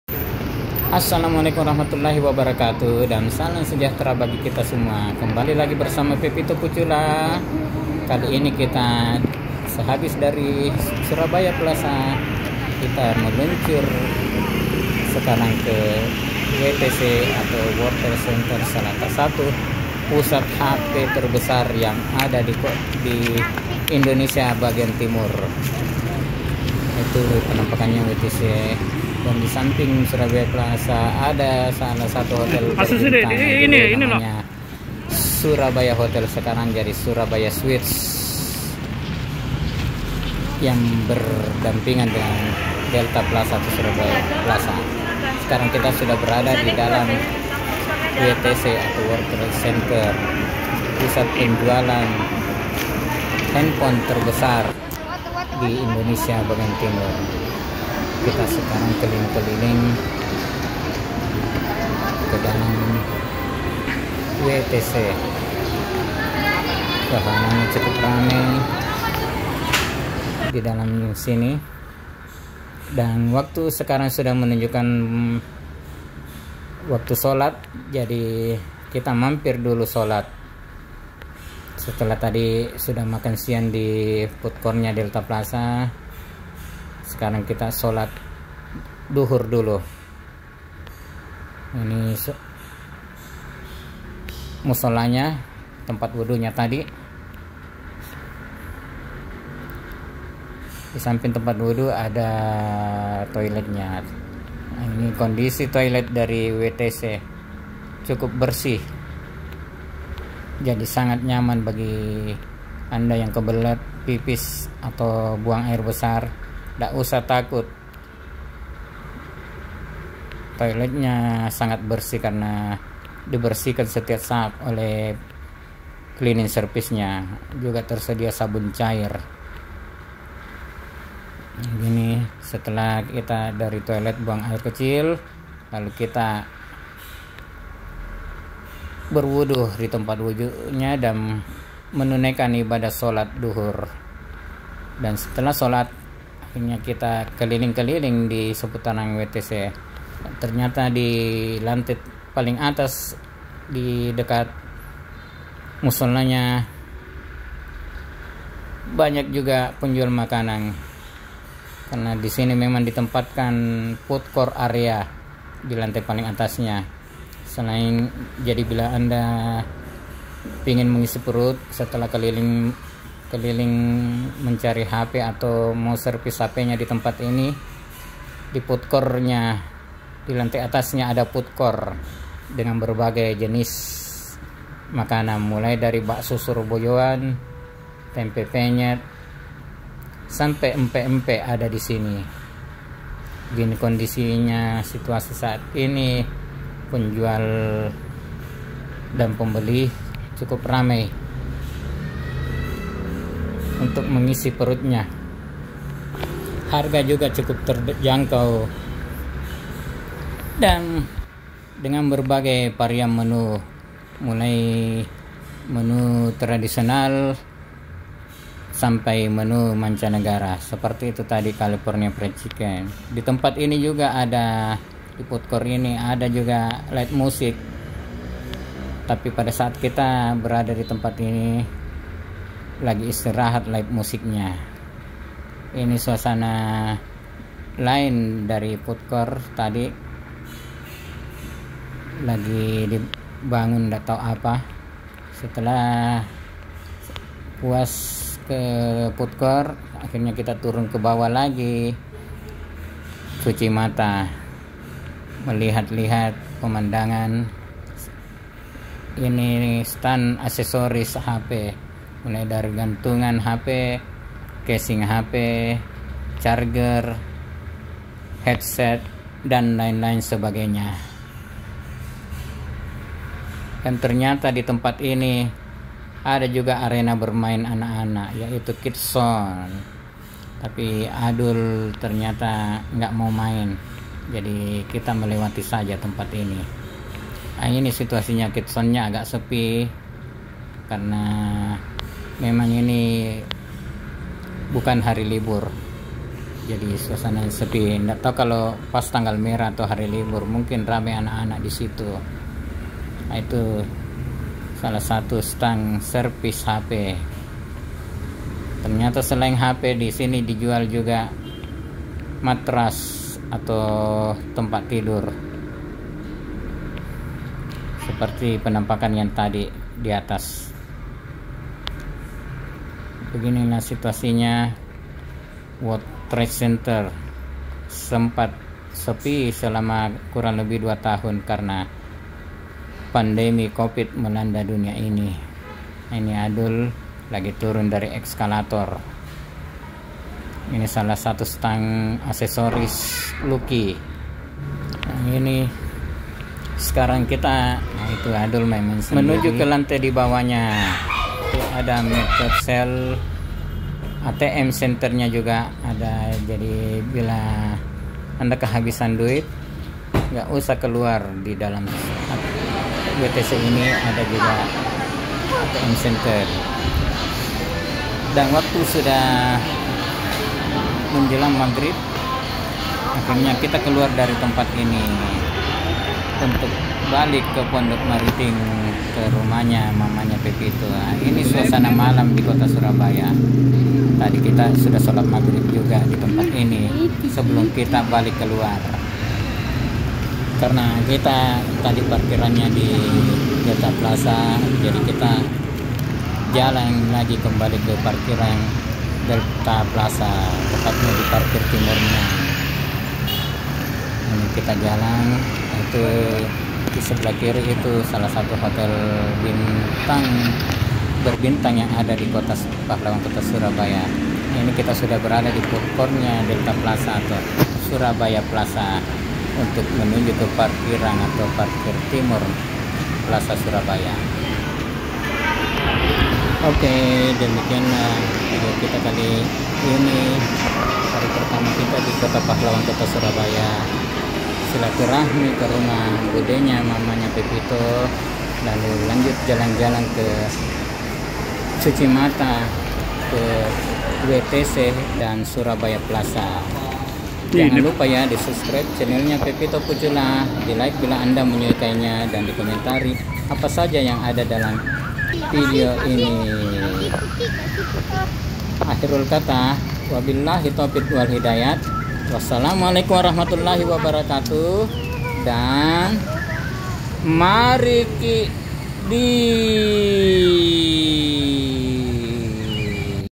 Cuci Mata di WTC Surabaya bersama Pepito
Habis dari Delta Plaza makan siang di foodcourtnya lalu jalan jalan ke WTc pusat Handphone di Surabaya yang bersebelahan Plaza Surabaya. Surabaya, Kamis 19 Mei 2022